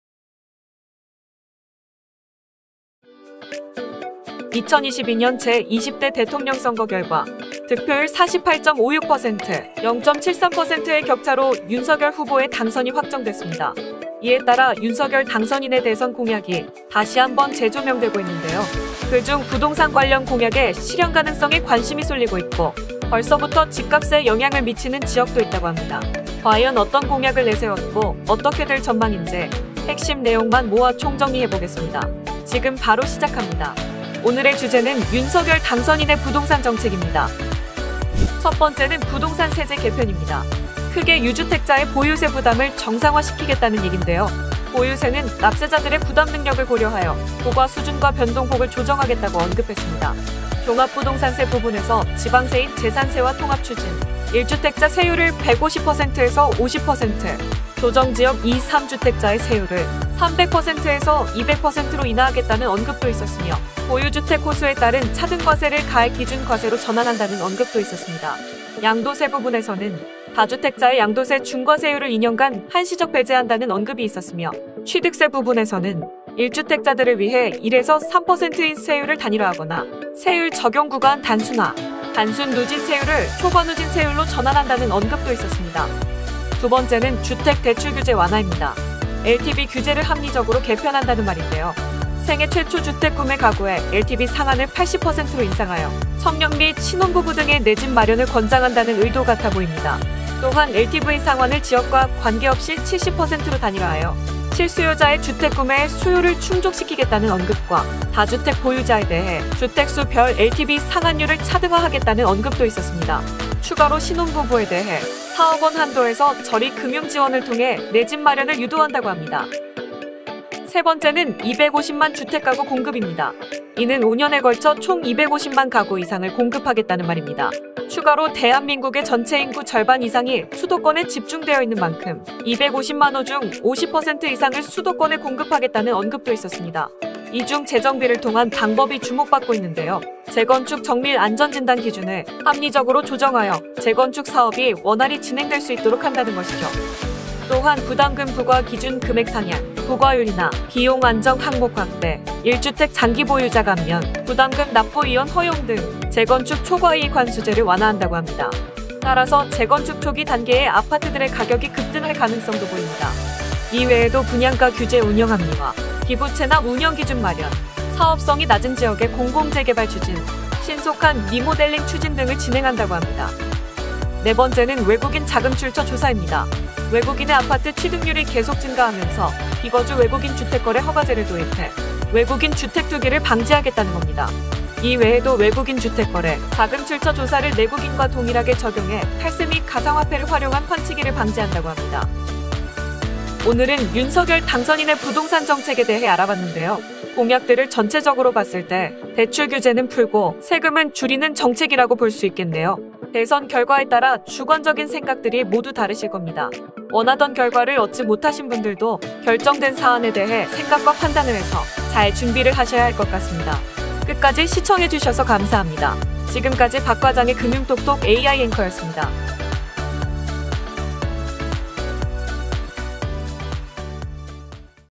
윤석열 부동산전망, 2022년 부동산 규제완화 총정리
이번 제 20대 대통령으로 윤석열 후보가 당선되셨습니다.
그에 따른 윤석열 당선인의 부동산 규제완화 공약이 재조명되고 있는데요!
어떤것들이 있는지 정리해봤습니다.
영상 끝까지 시청해주시고, 좋아요 구독 댓글은 저희에게 큰 힘이 됩니다🙏

00:00 - 인트로
00:40 - 유주택자 보유세
00:51 - 종합부동산세
01:09 - 양도소득세
01:14 - 취득세
01:30 - LTV규제 합리적 개편
02:05 - 주택가구공급
02:21 - 재건축 안전진단 기준 조정
02:36 - 재건축 초과이익 환수제 완화
02:54 - 외국인 자금출처 조사